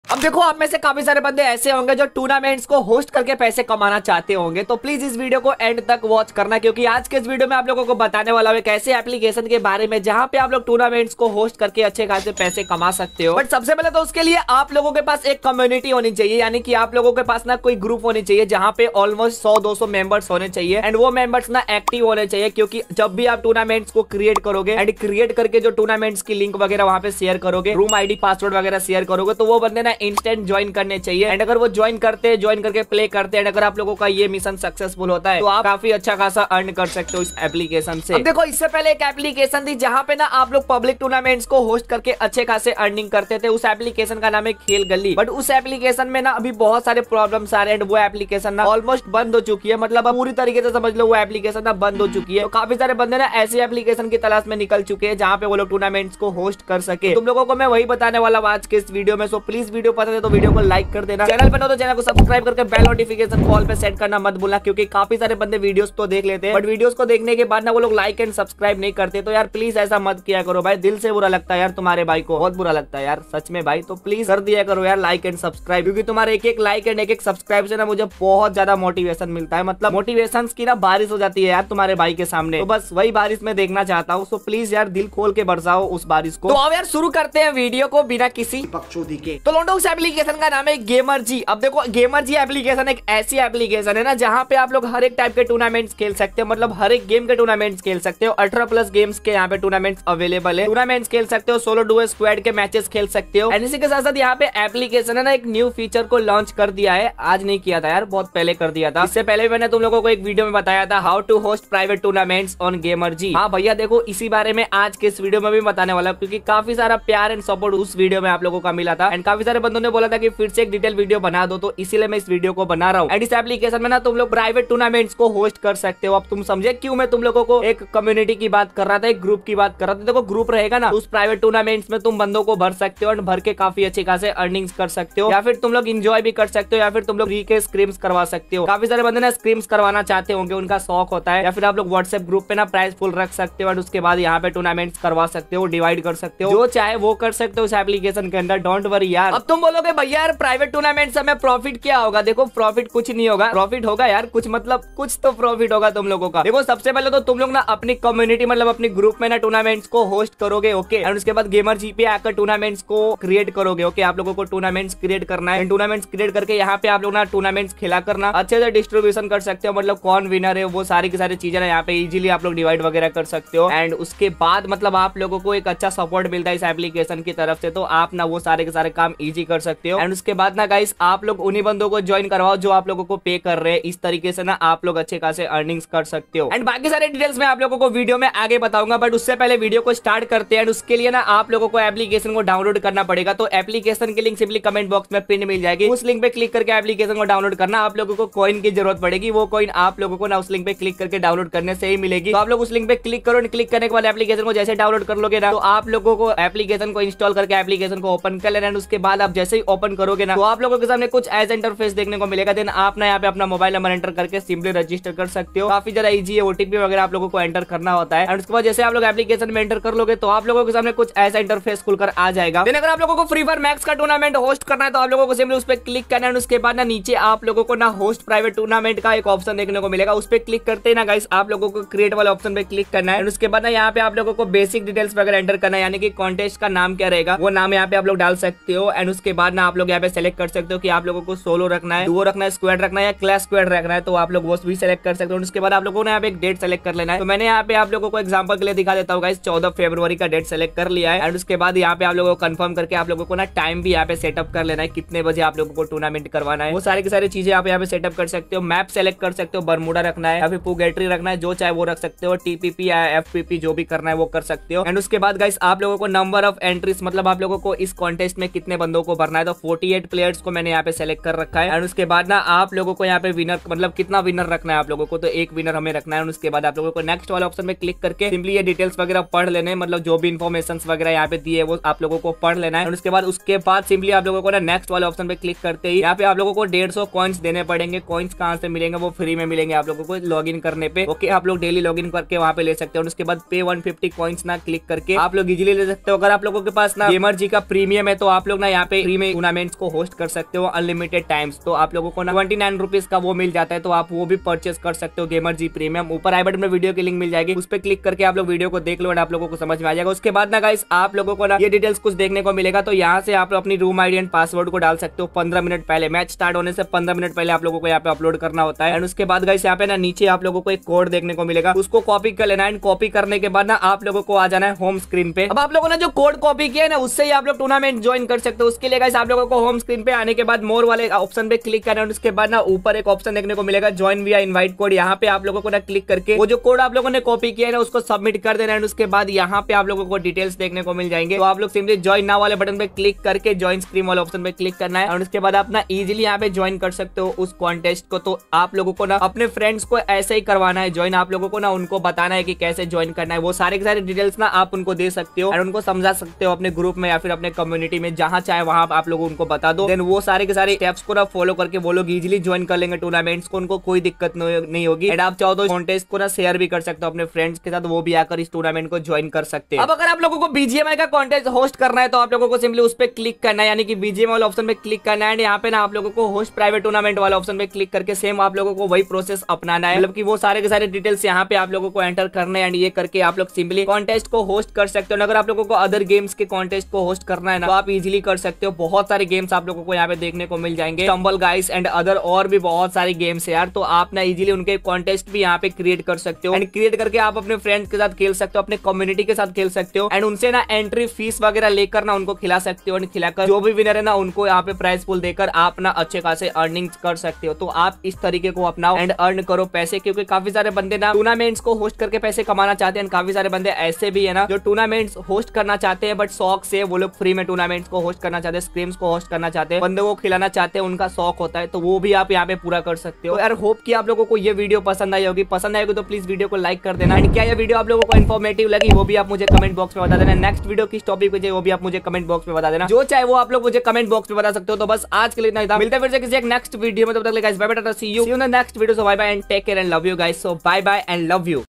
Host Free Fire And Bgmi Tournament And Earn Money 🤑 | Earn Money By Host Tournament In Application
Application links Is In the Comment Box